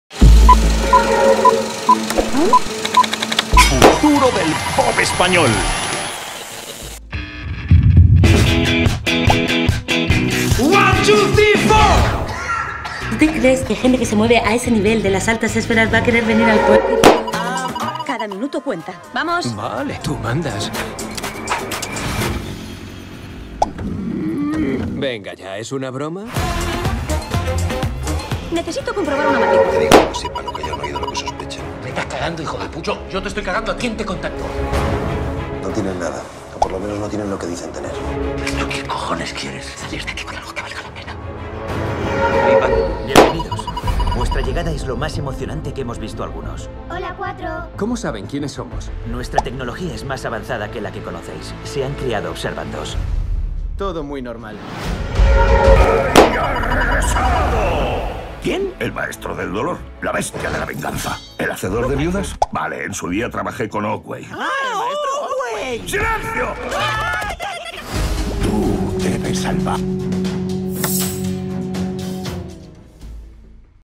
Taquilla España - 18 al 20 marzo de 2016
'Kung Fu Panda 2' finalmente vence a 'La serie Divergente: Leal' en la taquilla española.